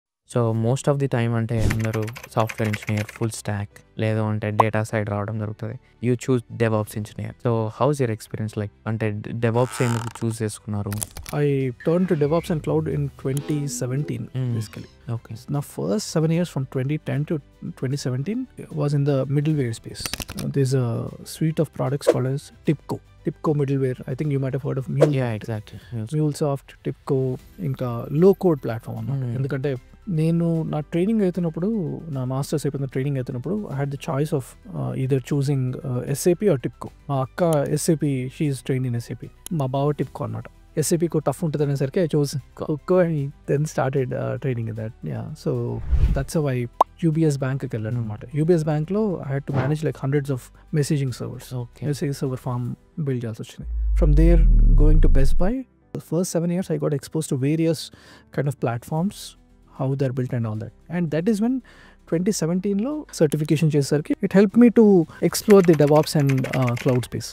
Why I Chose DevOps Over Software & Data Science | Arun Gunti with Anil Meda | Tech Talk
In this episode, host Anil Meda talks with Arun Gunti about his journey into the DevOps world — a field often overlooked compared to Software Engineering or Data Science.
Arun shares why he chose DevOps, what makes it exciting, and how his experience in automation, cloud, and CI/CD pipelines shaped his tech career.
If you’re a fresher or IT professional deciding your career path, this conversation will help you see why DevOps is the future of modern development.

🎧 Watch now to learn how DevOps is changing the tech landscape